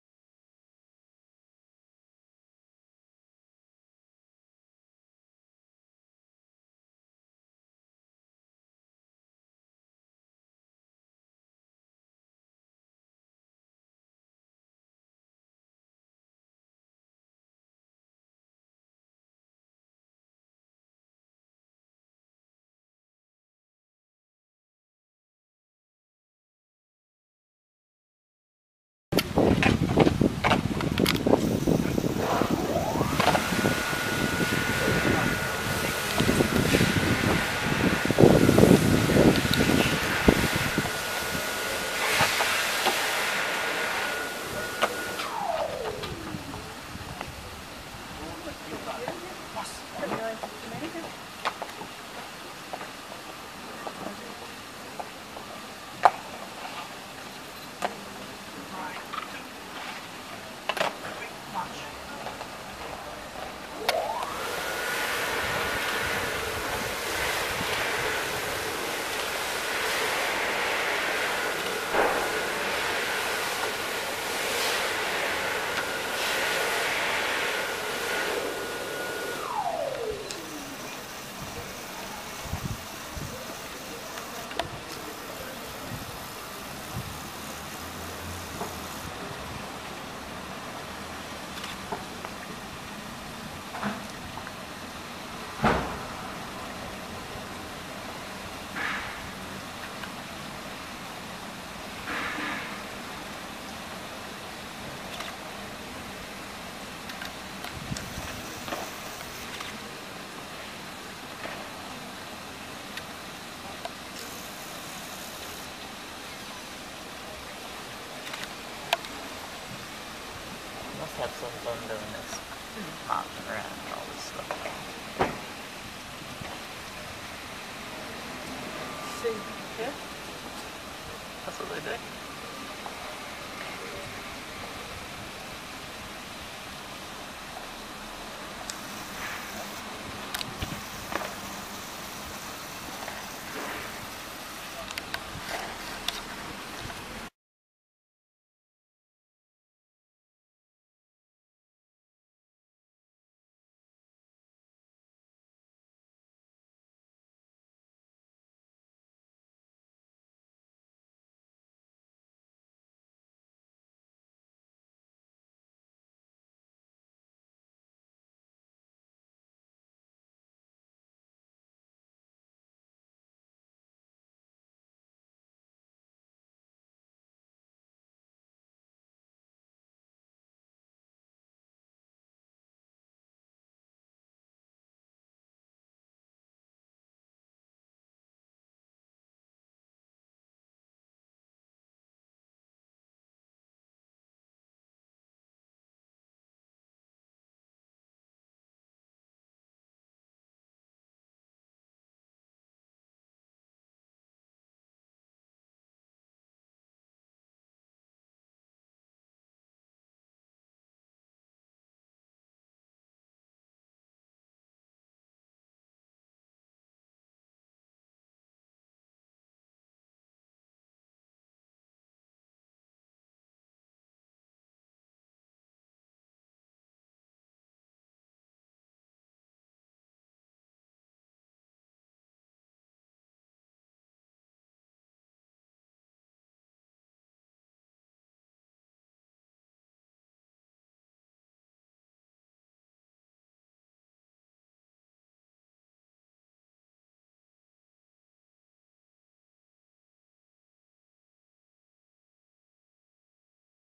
Edinburgh Visit to Holyrood Palace, 'The Changing of the Guard', PART III, By sheila May 2014
The Family Tree Trip in 2014 in Scotland has been so rewarding---the last time I visited this beautiful city was in 1976 with a college friend when we were Youth Hostelling around Scotland I definitely want to return---so much to explore and see in this grand old city and loads of ancestral links as well.